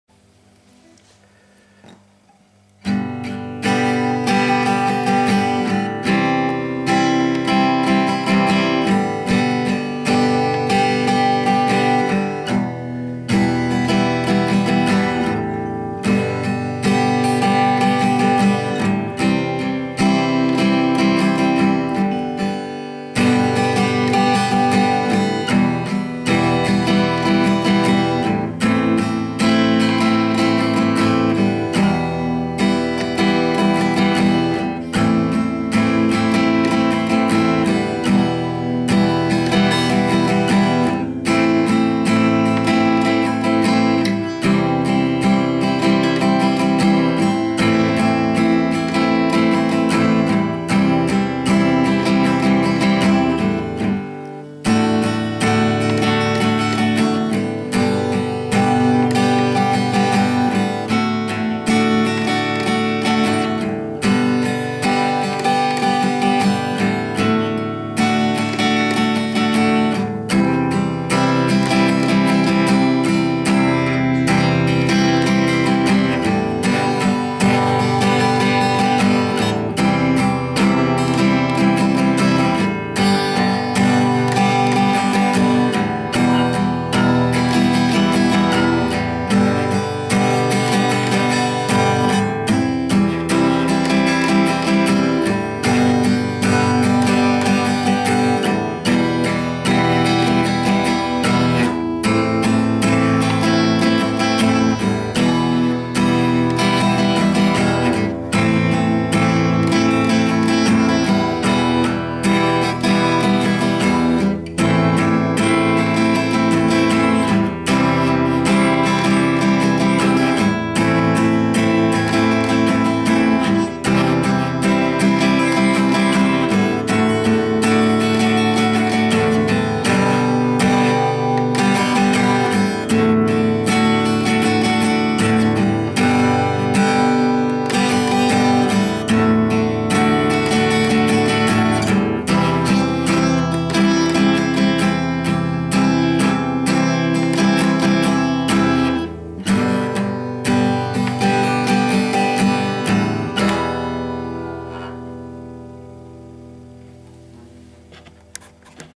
essai compo
petit extrait d'une compo sur laquelle je suis en train de bosser merci de me donner vos avis